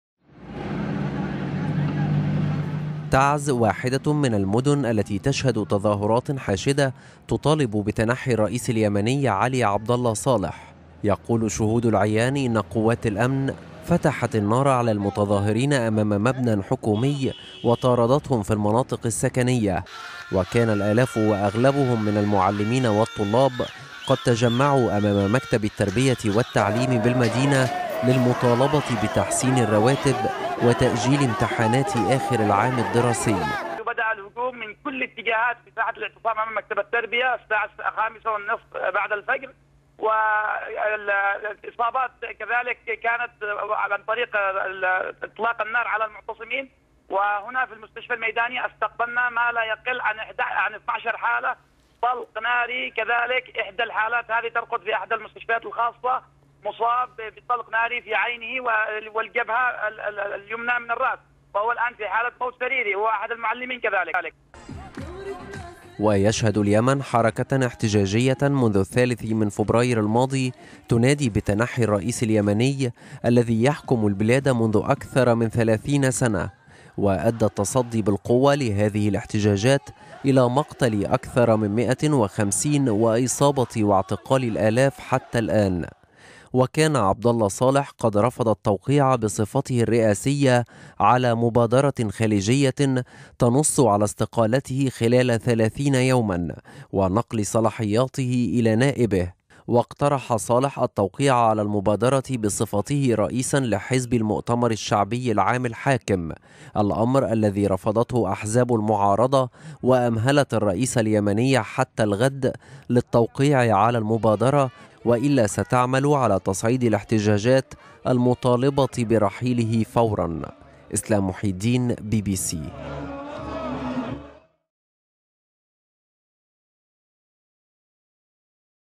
قتيلان في صدامات بين متظاهرين وقوات الأمن اليمنية
قتل اثنان من المعتصمين في مدينة تعز جنوبي صنعاء حين داهمت قوات الأمن اليمنية آلاف المدرسين المعتصمين أمام مكتب التربية.